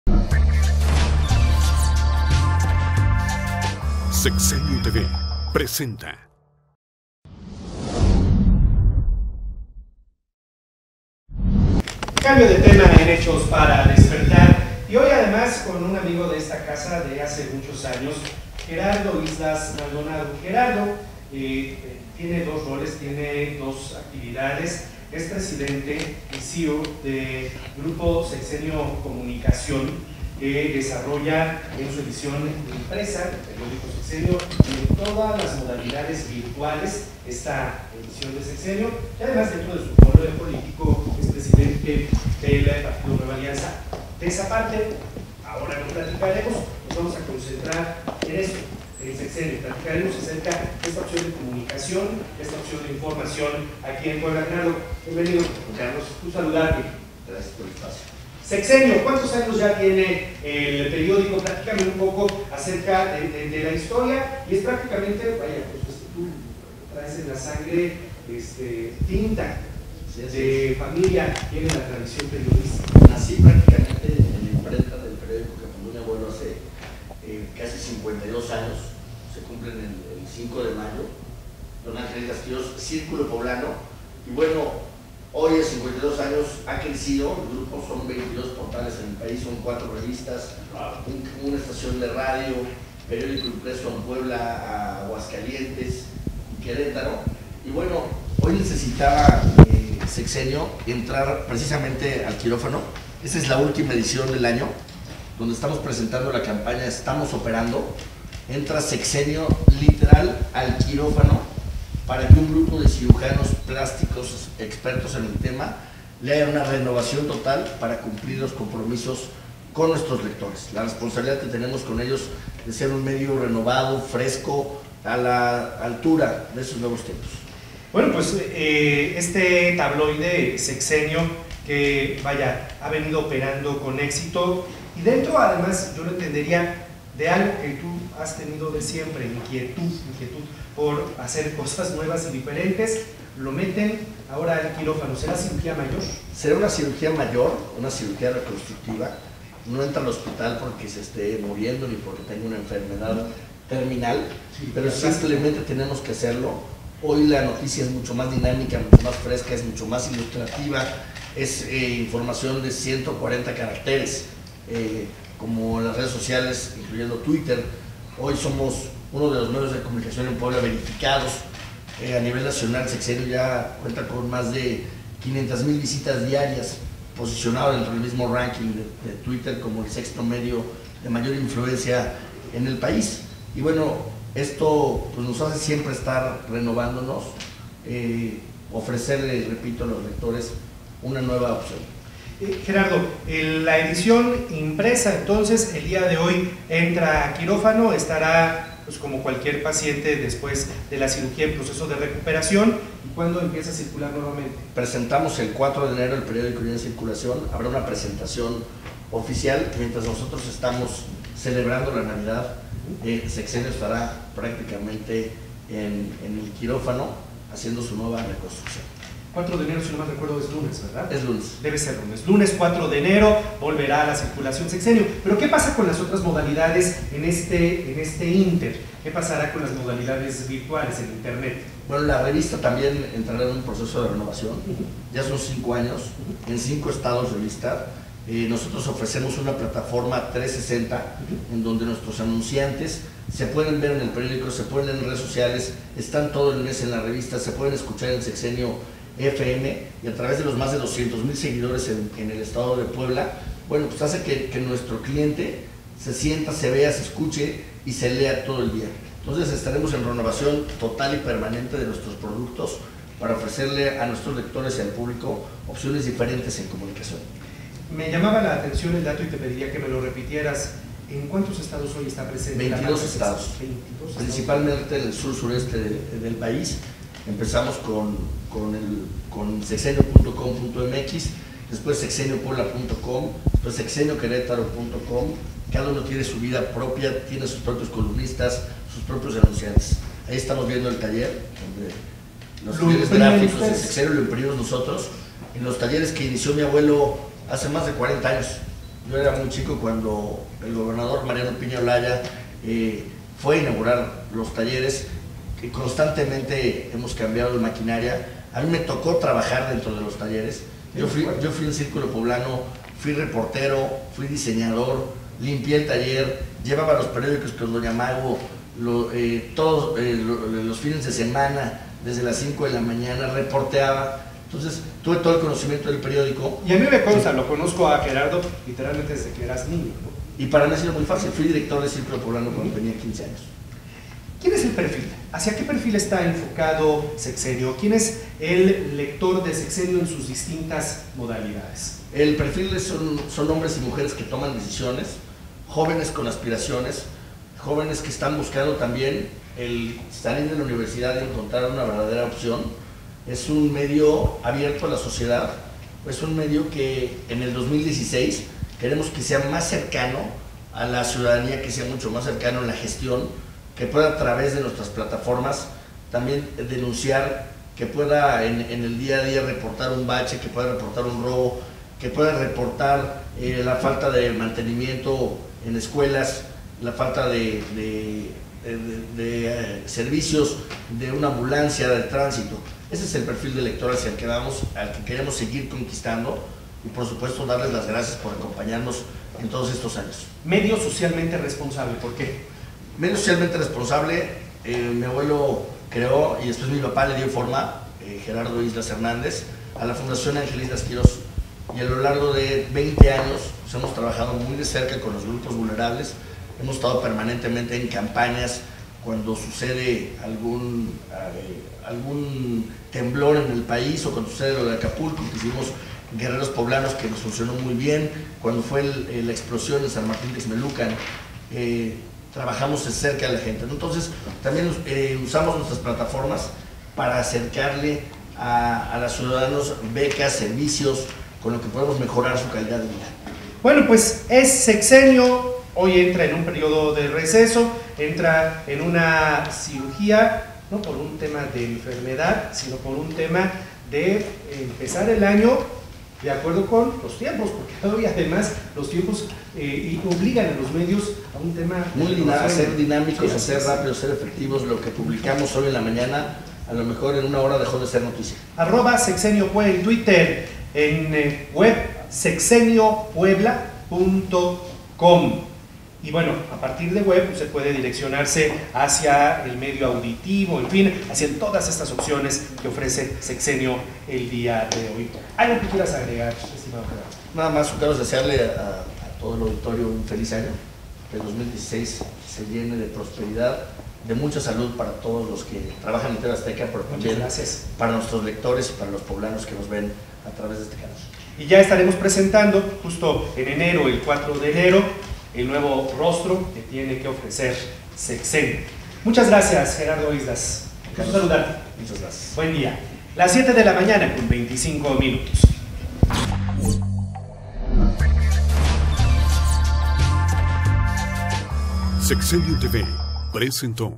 En 2016, Sexenio impreso será más cercano a la gente: Gerardo Islas
A partir de este día Sexenio impreso entra al quirófano, por lo que dejará de circular en lo que resta de año y el próximo 4 de enero nuevamente estará en circulación, sólo que será con un nuevo diseño.

En entrevista en TV Azteca Puebla, el presidente de Grupo Sexenio Comunicaciones, Gerardo  Islas Maldonado, destacó que en el año entrante los ciudadanos podrán tener una mayor participación en este medio informativo que ha logrado posicionarse como uno de los más leídos en Puebla.